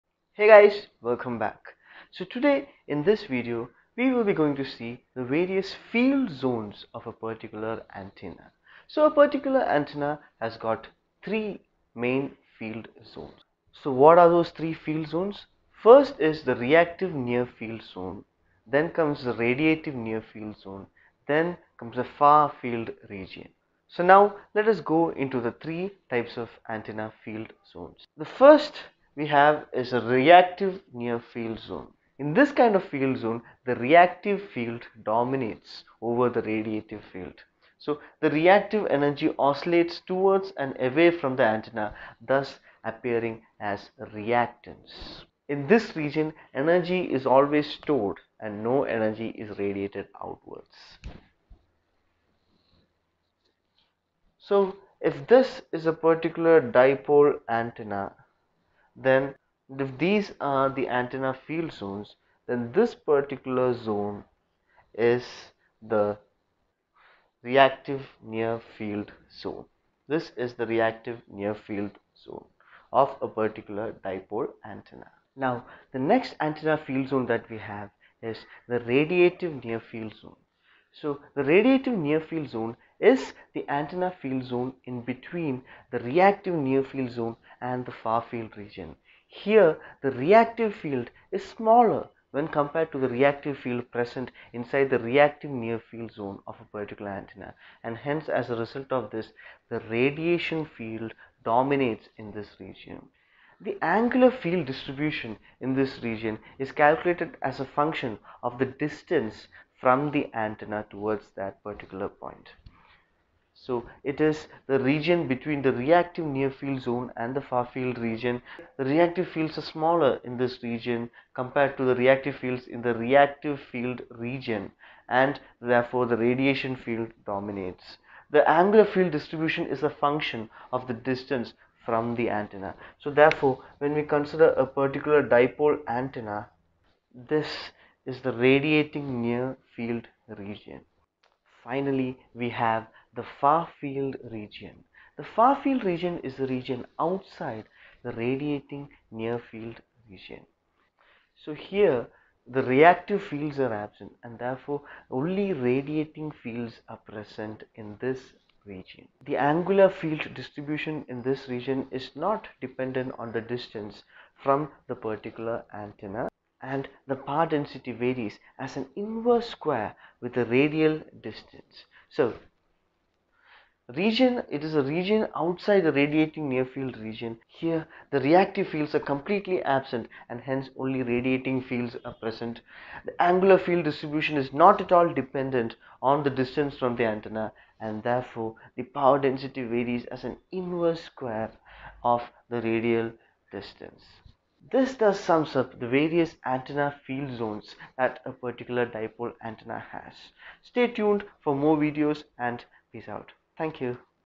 Antenna Field Zones | AWP Module 1
EC306 - Module1 - Antena & Wave Propagation
This video gives a clear idea of the various field zones of a particular antenna!
Syllabus based on KTU!
Session taken by Rishi Remju.